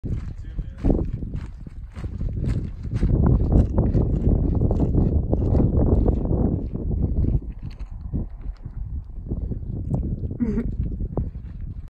Hiking the Highline Trail in MT
With Kev, Kayla, Morgy, & Nick at Glacier National Park, Montana